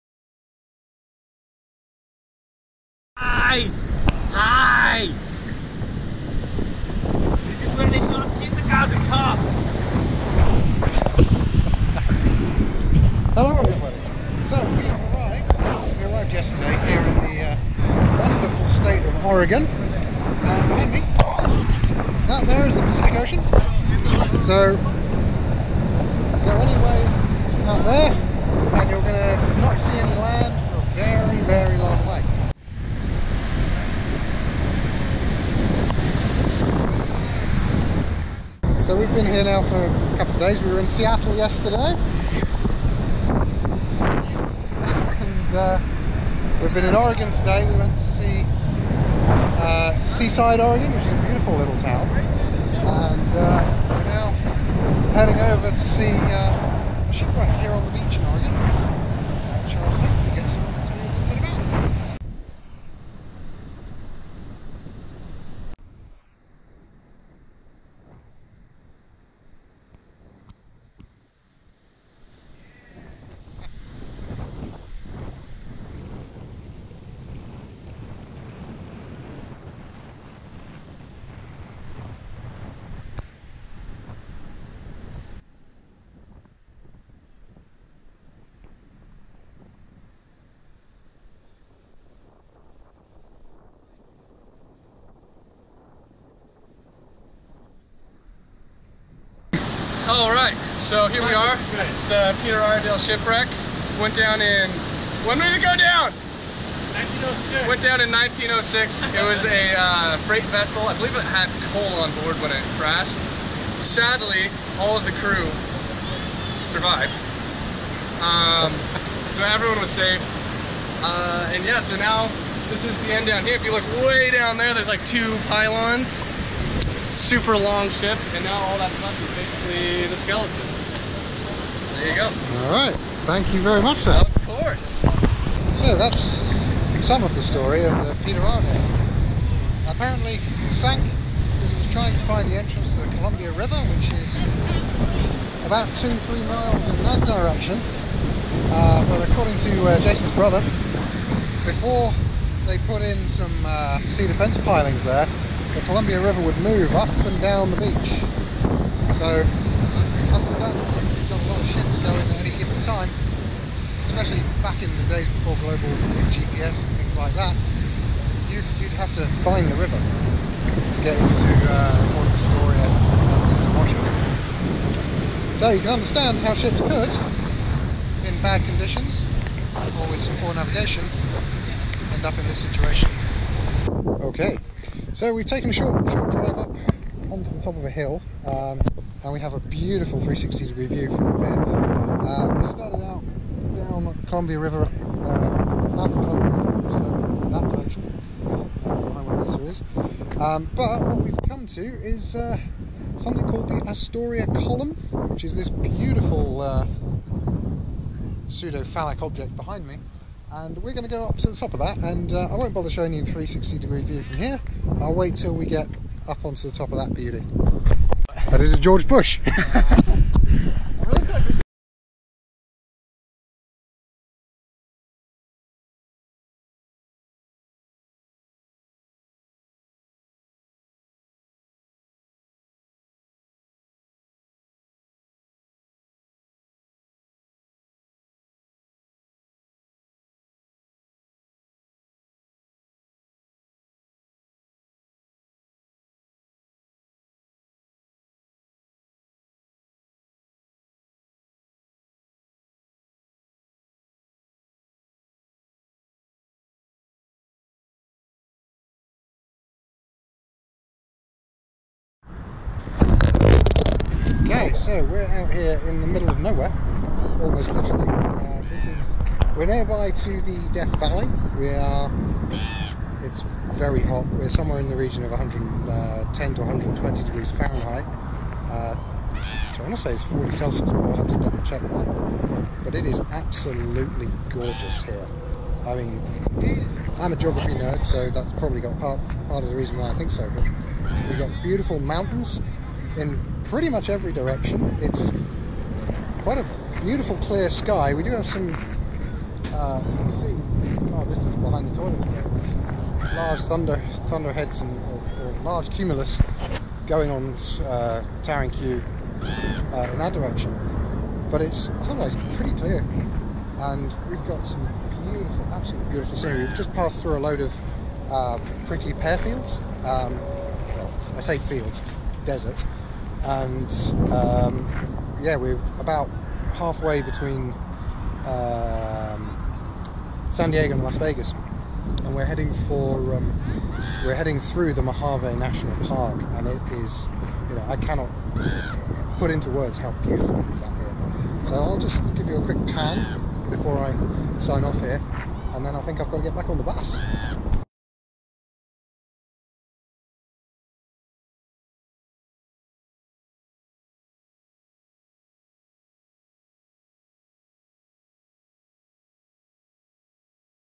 Anglo-American Road Trip Vlog 4 - From Oregon to the Mojave!
Drinking game for this episode: drink a shot for every use of the words "Beautiful", "Wonderful" or any derivative thereof.

After a short stay in Seattle, which was a sleepless one, before you ask... we travelled down to Oregon where one of Tom's friends (Jason) very kindly showed us around the wonderful Pacific Northwest.

Unfeatured in this video, We travelled through the Redwood Forests of Oregon and Northern California before making a day stopover in LA and San Diego.

Finally we made out way to the majestic Mojave desert, which I alone of our company seemed to find beautiful. However 120F heat probably doesn't help endear a place to you...